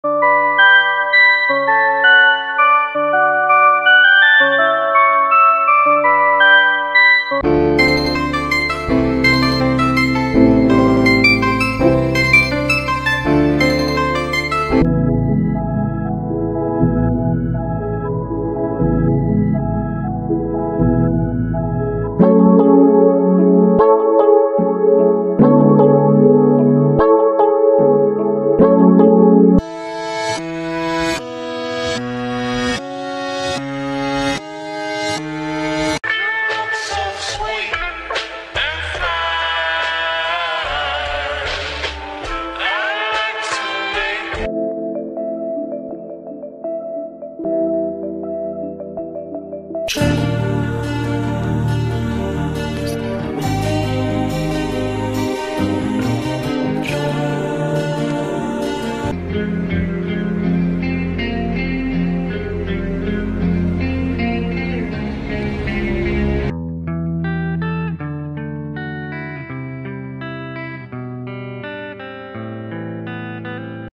Free Sample Pack for Producers |Link in Description
This free sample pack contains 10 samples, 5 are made and the other 5 are sample finds. Let me know what type of packs yall want next.